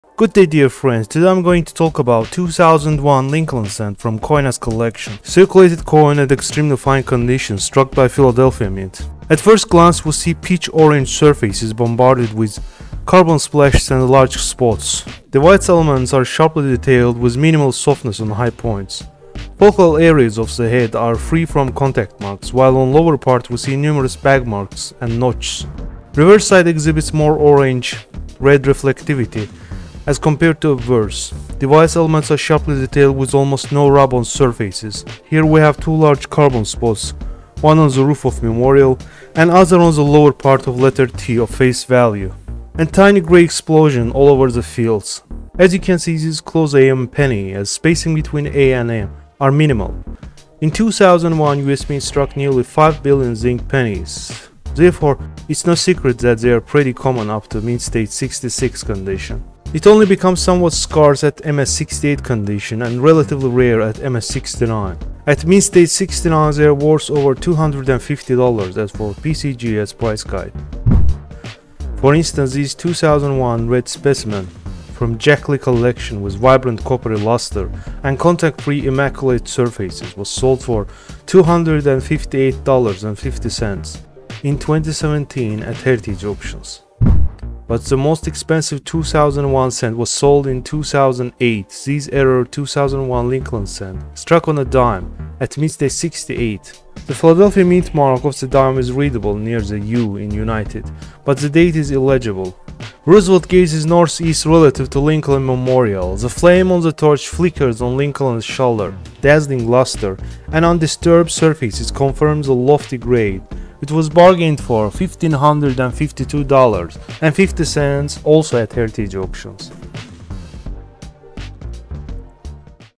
RARE 2001 PENNIES WORTH BIG MONEY
In this video, I'm sharing with you some rare and valuable 2001 pennies that are worth big money!

If you're looking for rare pennies to add to your collection, then you'll want to check out this video. In it, I'm sharing with you some 2001 pennies that are worth a lot of money. If you're interested in coin collecting, then you'll want to watch this video!